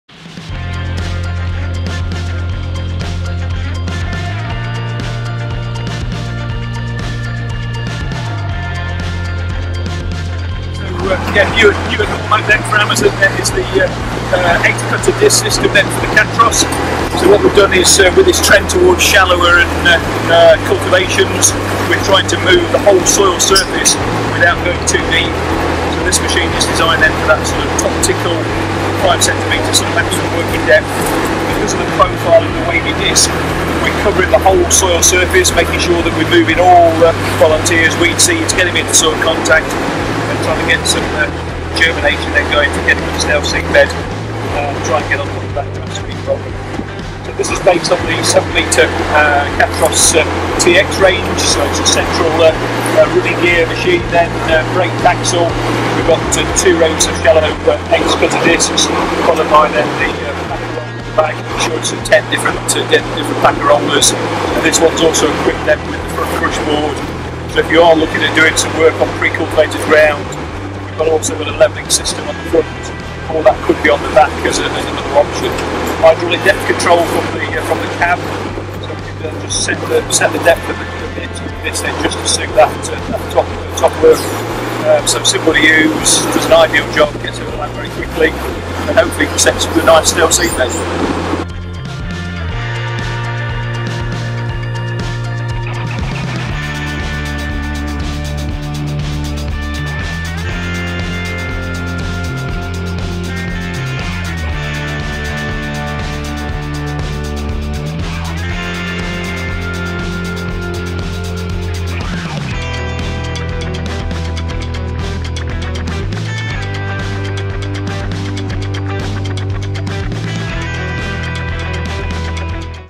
Amazone Highlights from the NORMAC Cultivations Demonstration Day 2021
On 8th September 2021 the Farmers Guide team headed to the NORMAC Cultivations Demonstration day in Norfolk. Run by the Norfolk Farm Machinery Club, the day allows visitors the chance to see the top cultivation machinery manufacturers in action, and more importantly, in person.

We caught up with Amazone Ltd managing director Simon Brown, to discuss the new Catros+ 7003-2TX, having launched at Cereals this year. A fleet of G&J Pecks JCB Fastracs was used to demo all equipment.

Use our Linktree to find us on all major social media sites;